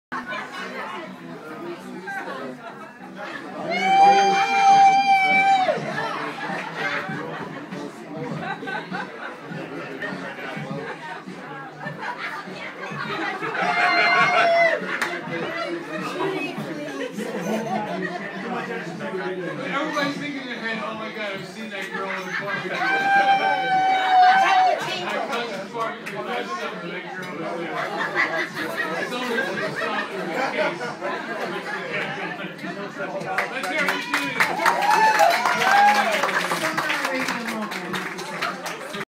Dennis & Judi Diner Tour: Pantagis Diner in Edision
Judi does her "drink girl" impression.